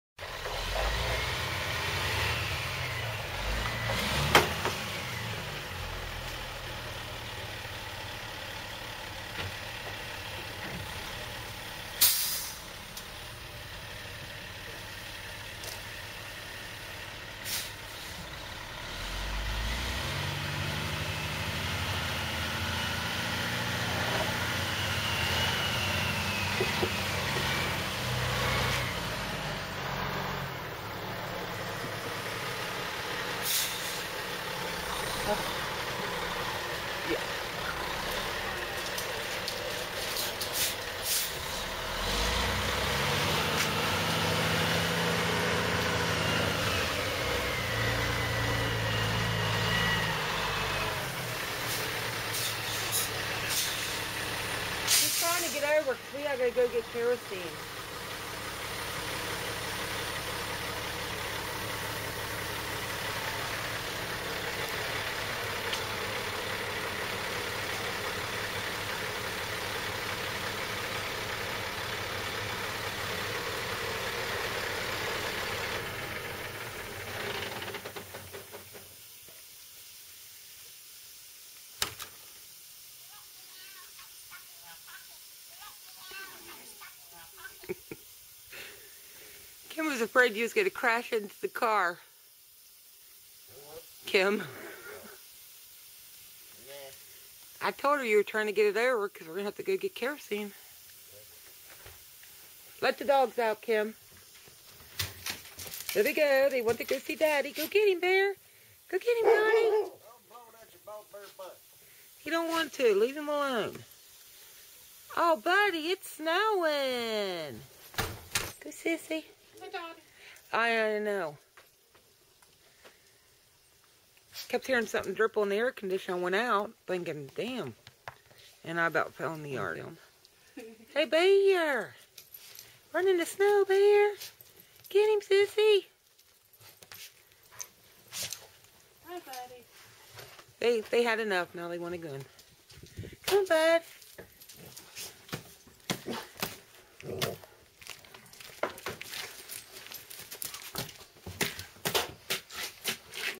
Snow and Milk truck
A short video of Steve, his milk truck and our dogs  in the snow.